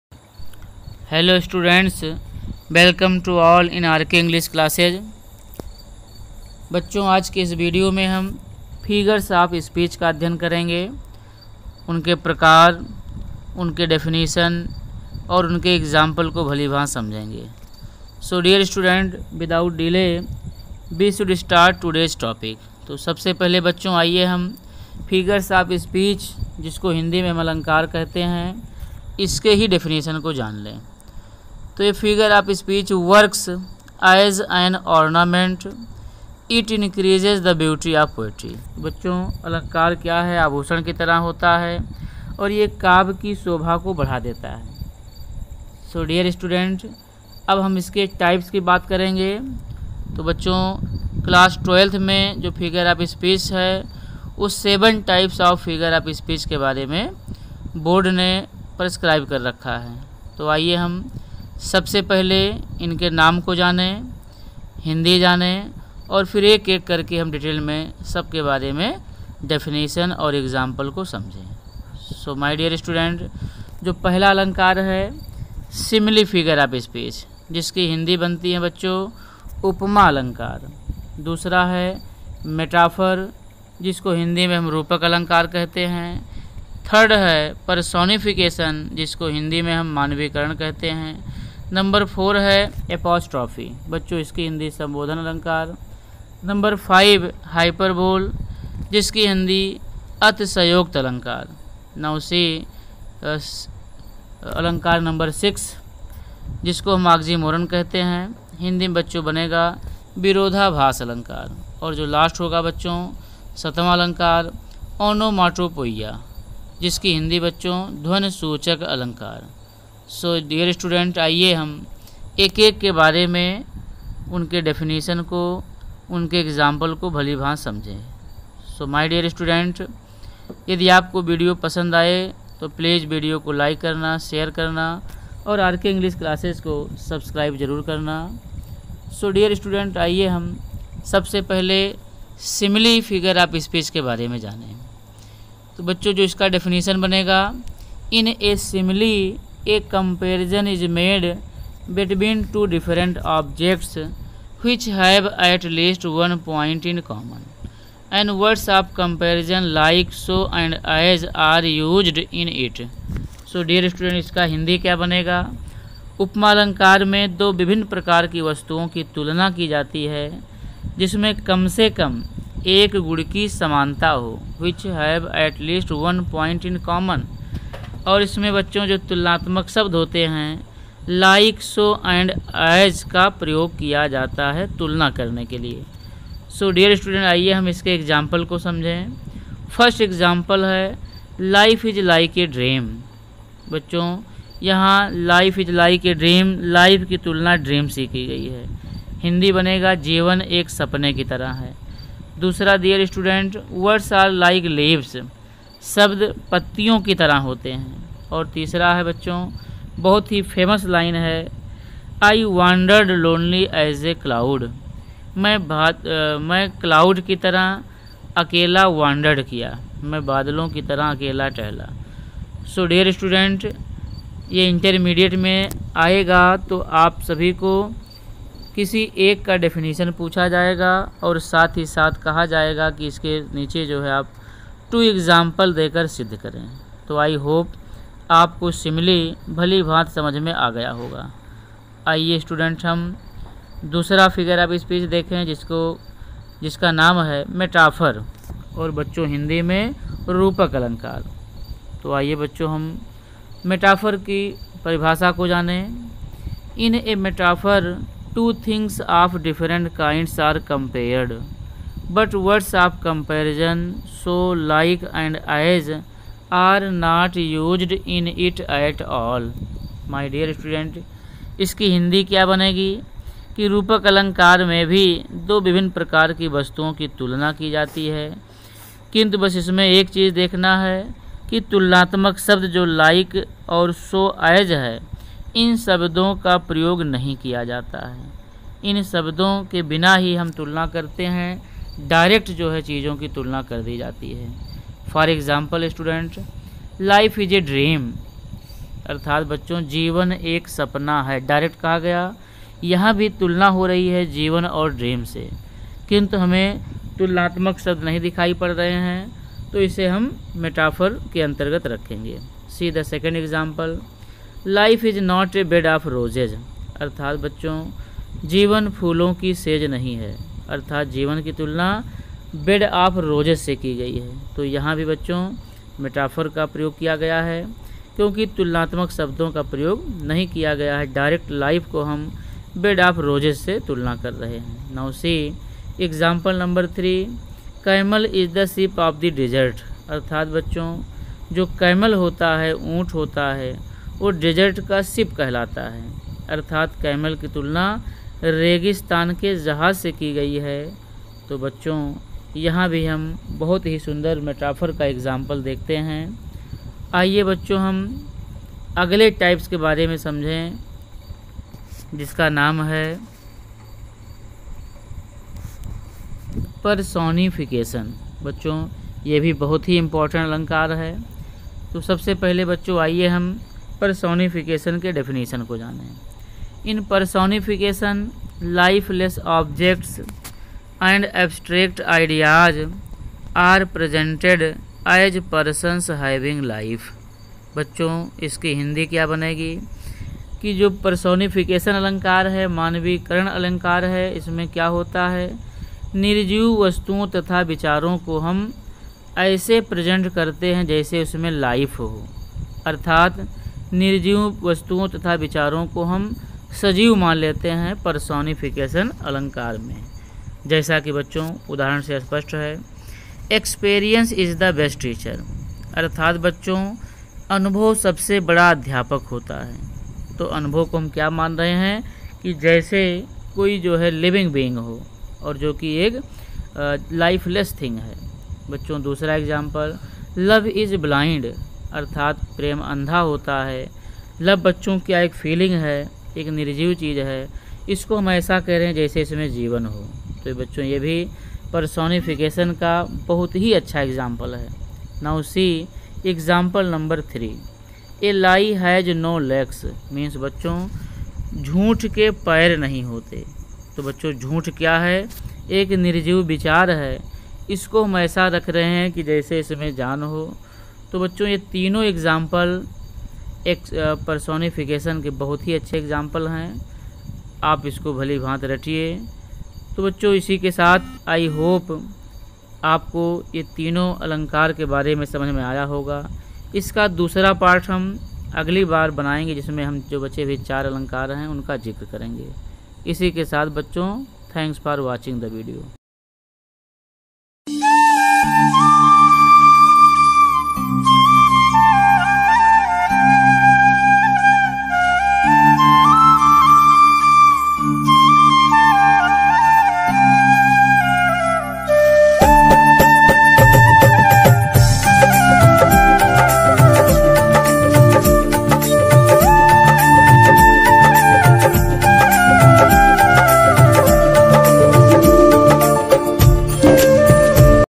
Figures Of Speech=Part=01 ✳️For all competitive Exams+Class-11,12
Figures Of Speech//Part=01Simile,Metaphor, Personification, Apostrophe Definition and examples with hindi for all competitive Exams and Class-12 by Dr. Raghunath Sir